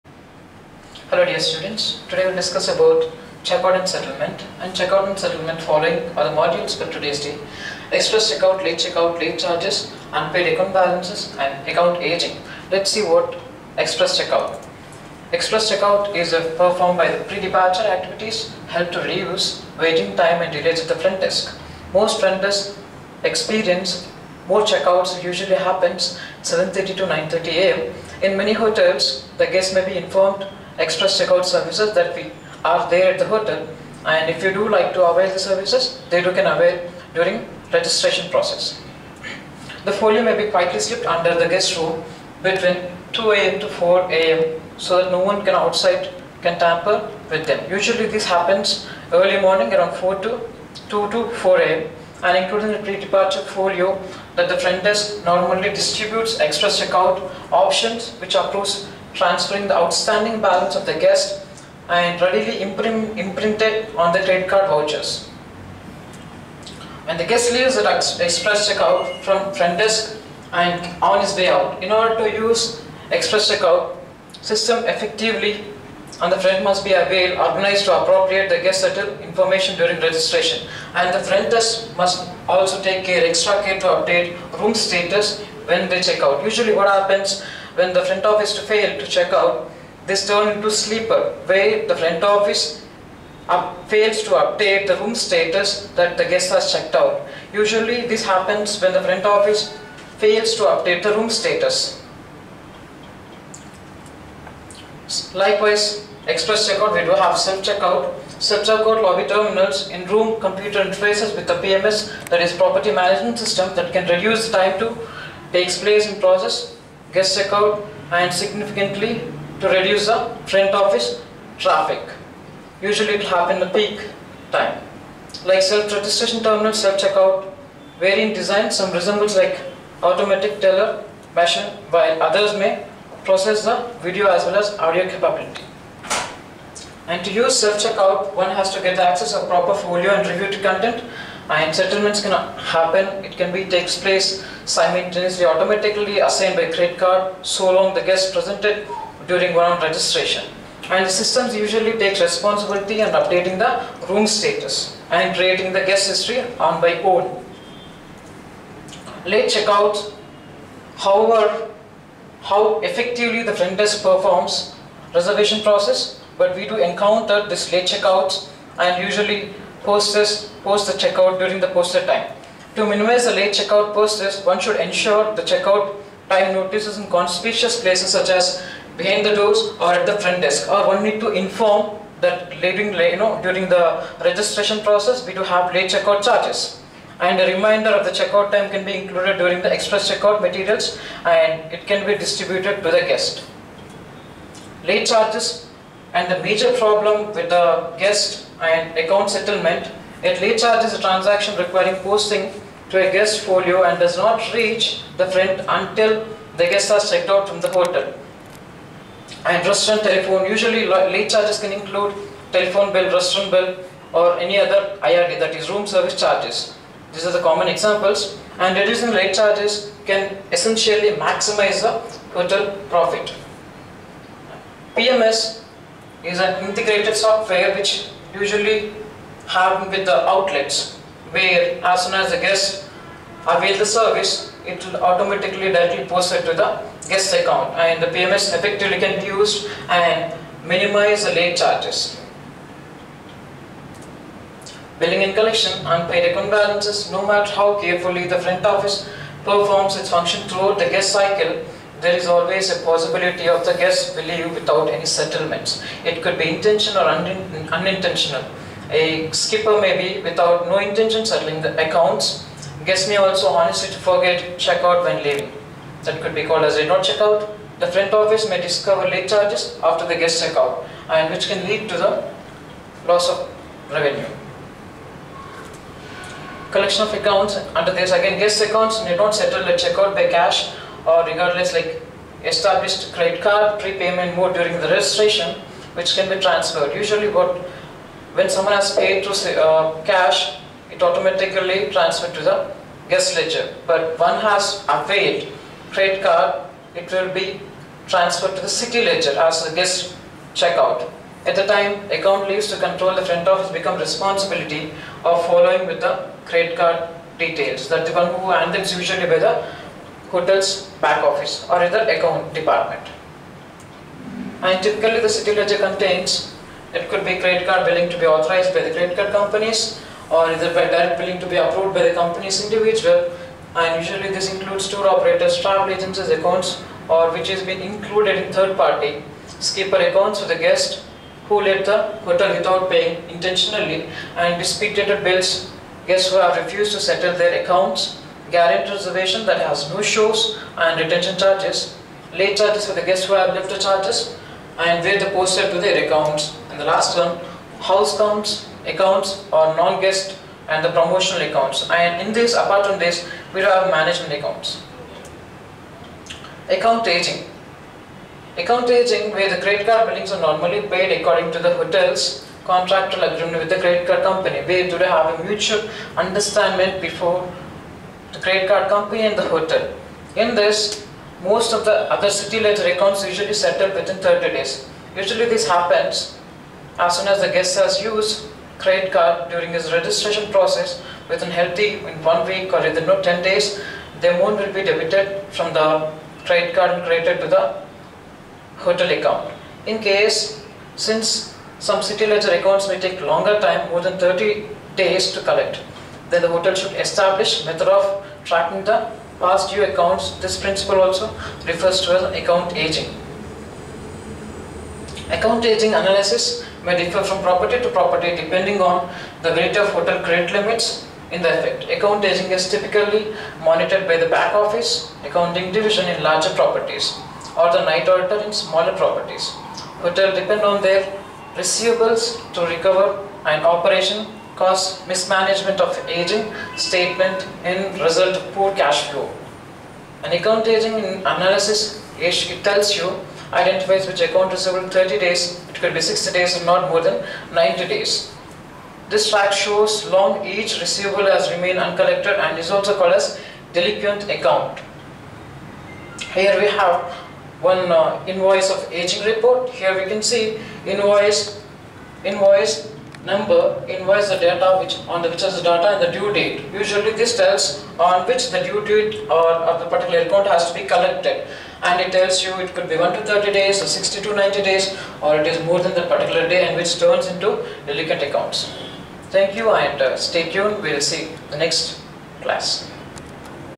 4th Semester UNIT - III Check Out Procedures of Front Office Operations on 17th March 2020.
B.Sc in Hotel Management 4th Semester  of Front Office Operations.